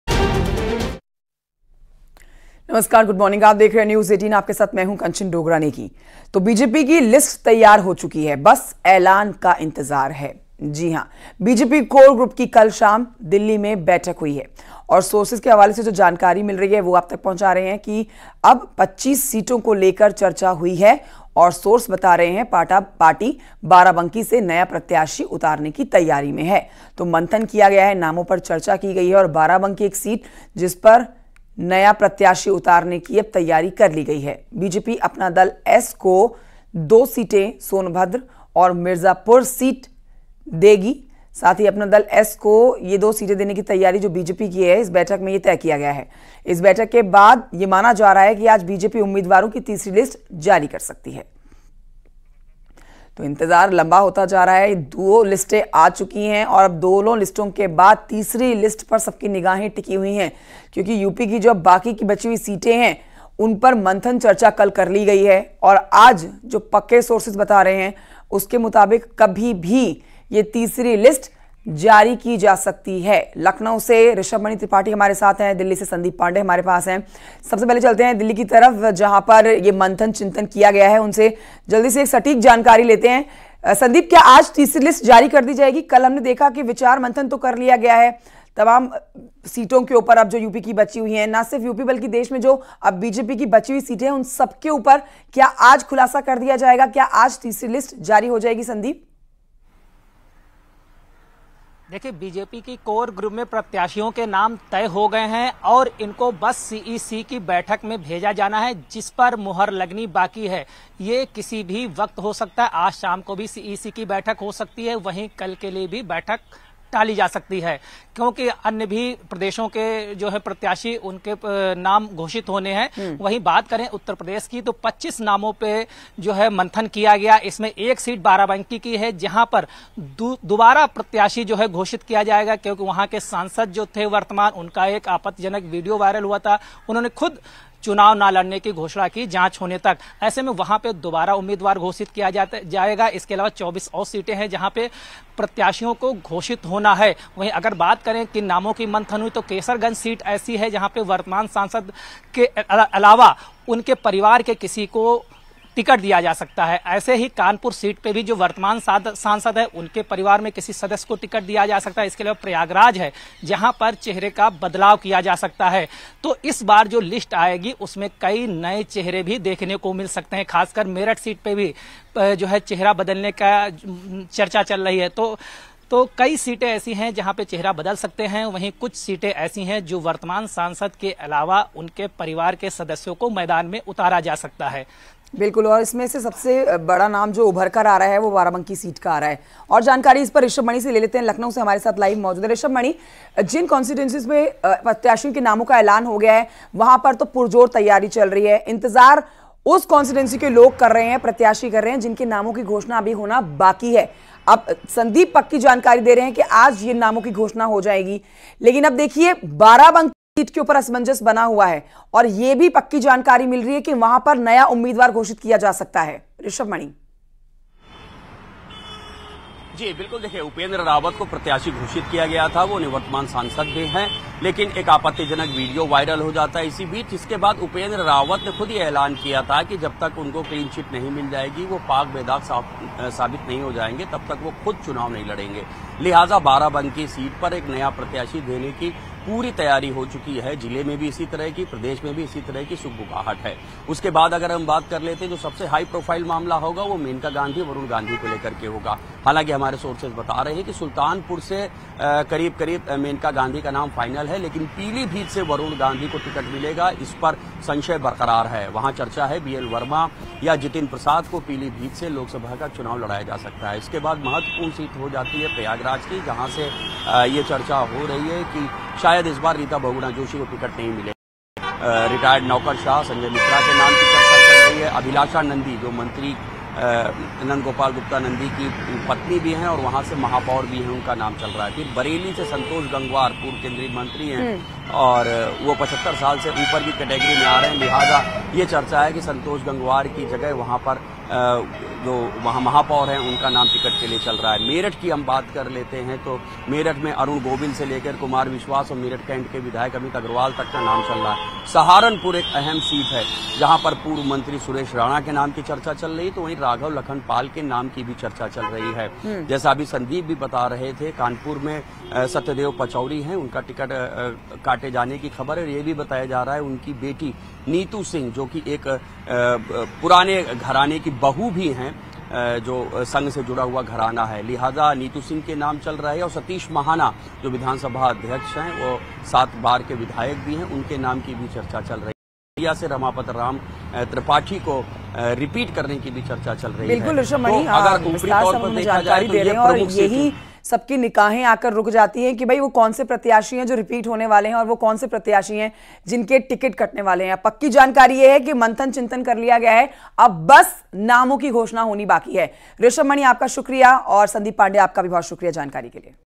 Loksabha Election 2024 : UP BJP के कोर ग्रुप की हुई बैठक, उम्मीदवारों पर 'मंथन' जारी | CM Yogi
भाजपा ने भी राजधानी दिल्ली में पार्टी के यूपी ईकाई के कोर ग्रुप की मीटिंग बुलाई है। माना जा रहा है कि पार्टी इस बैठक के बाद यूपी के लिए कई नए उम्मीदवारों का ऐलान कर सकती है।